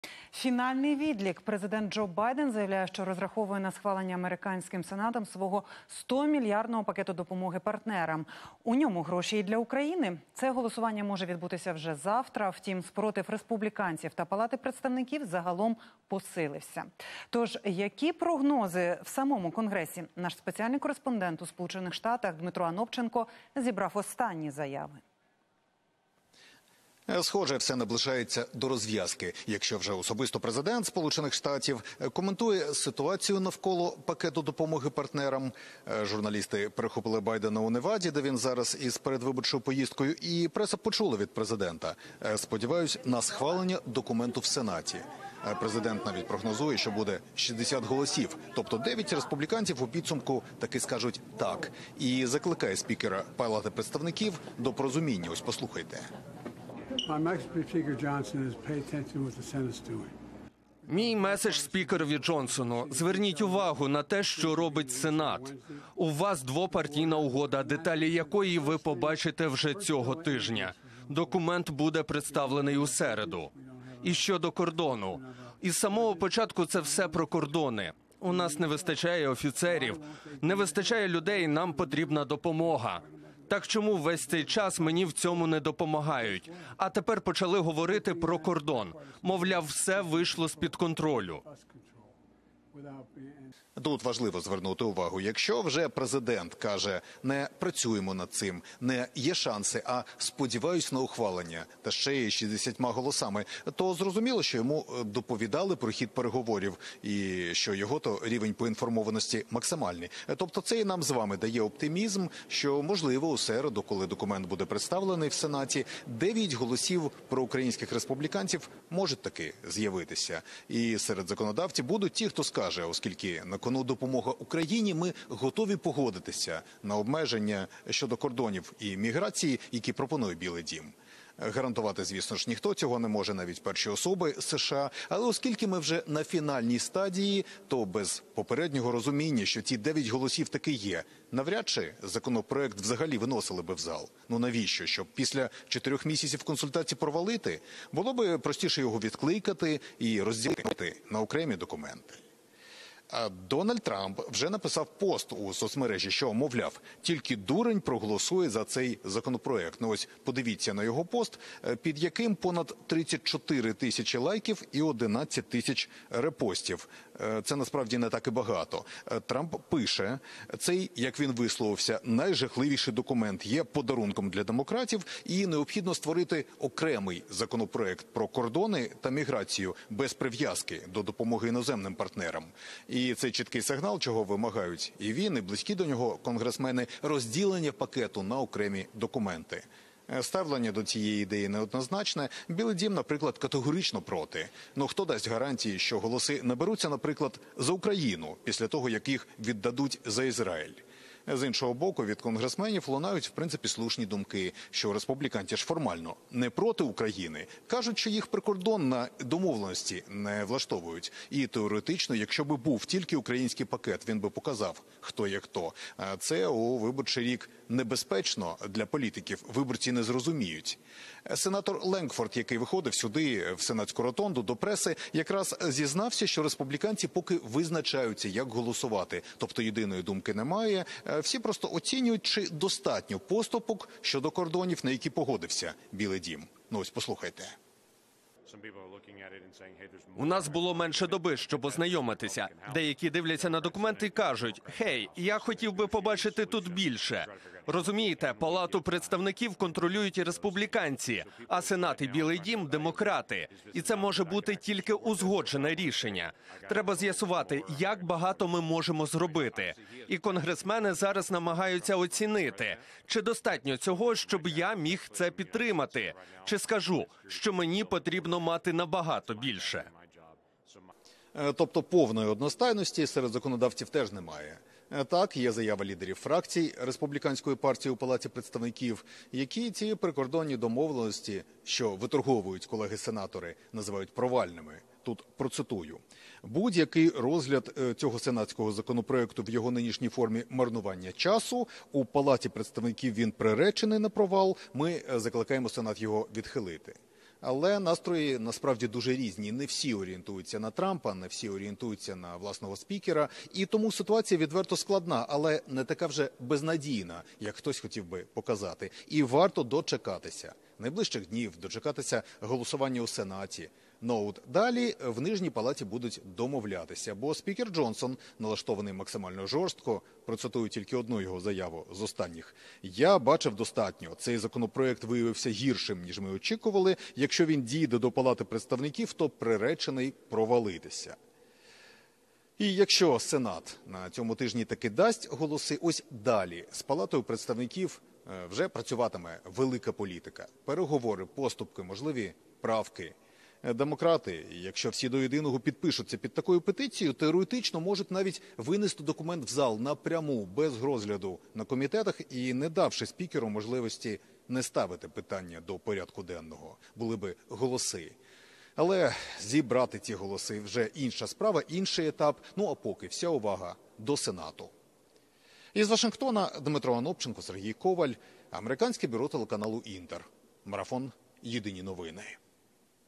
Байден анонсував схвалення пакета допомоги Україні: що може завадити
Це голосування може відбутися вже завтра. Втім, спротив республіканців та Палати представників загалом посилився. То ж, які прогнози в самому Конгресі?